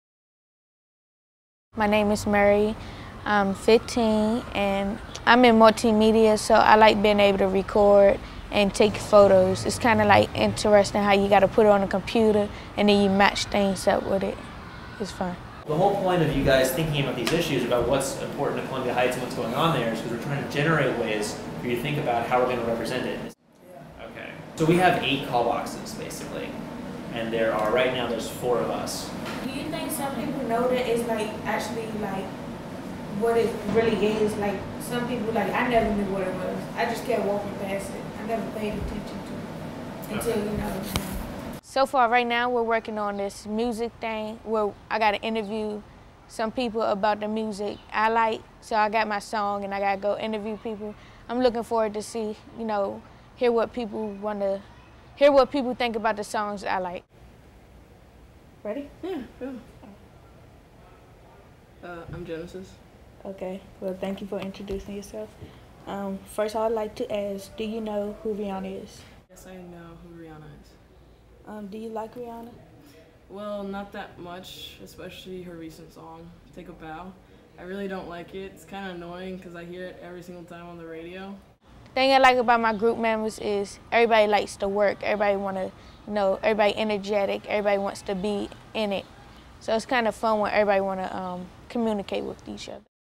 Summer Video Dailies #4
Mary discusses her work on the multimedia team.